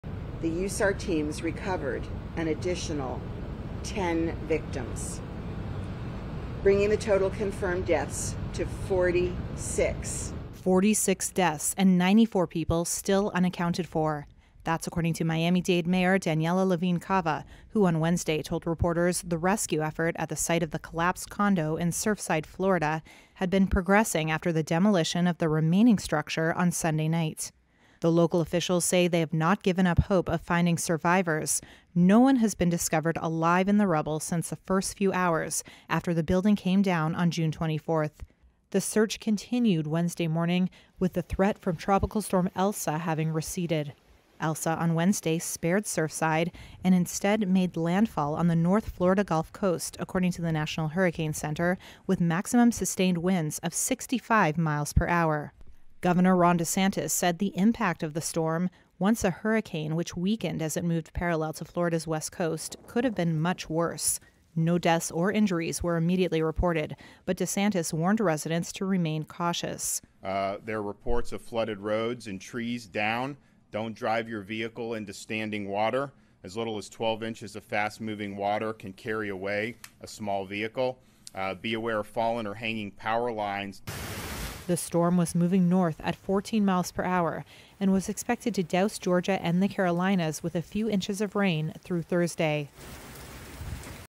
Death toll in Florida condo collapse rises to 46
Search and rescue workers recovered 10 more bodies from the rubble of an apartment block outside Miami that collapsed last month, bringing the death toll to 46, as hopes faded that any of the 94 people still unaccounted for would be found alive.